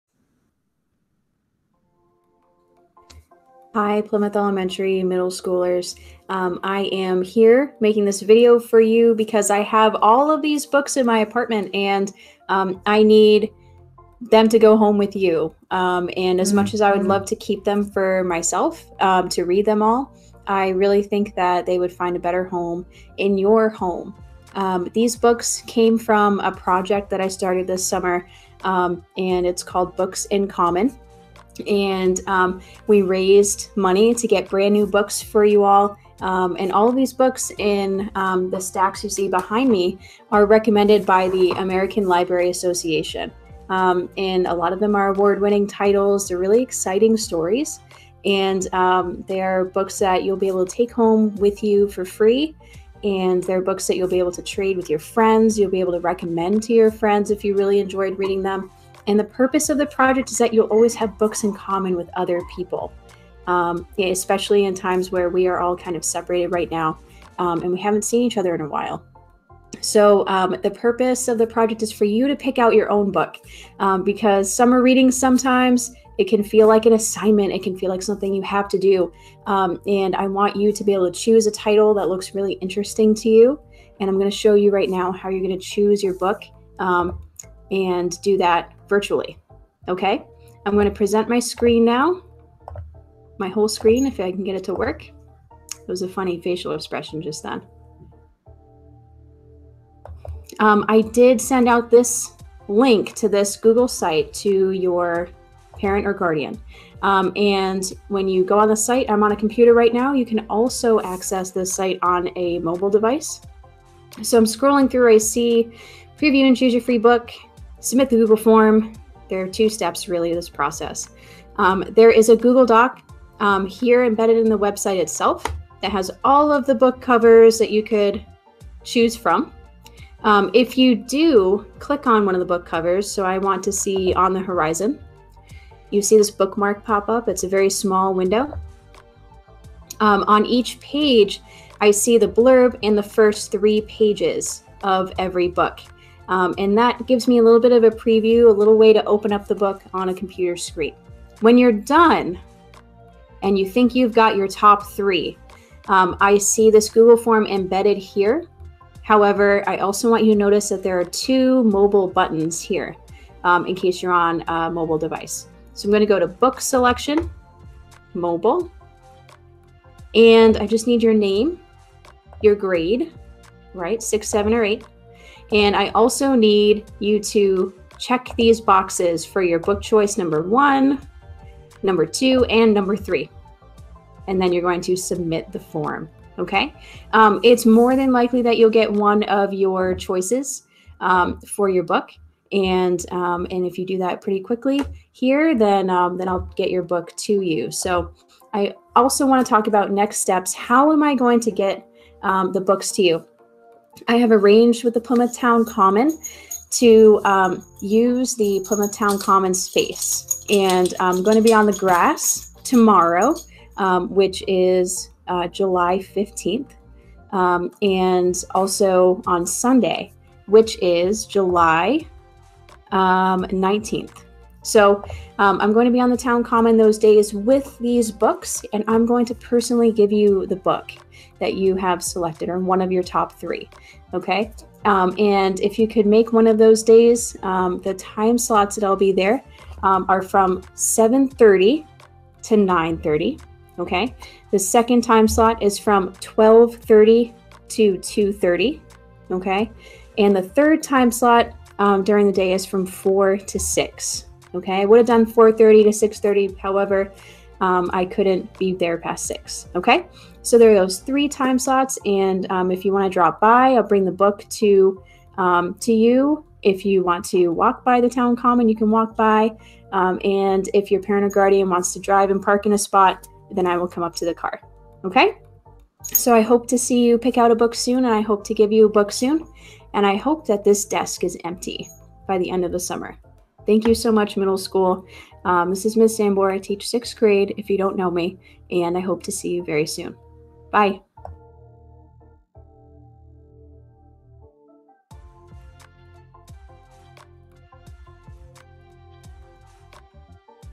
FREE Books for PES Middle Schoolers - Books in Common Project
Students grades 6-8 will have consistent opportunities to visit our local town common and take one to two books home.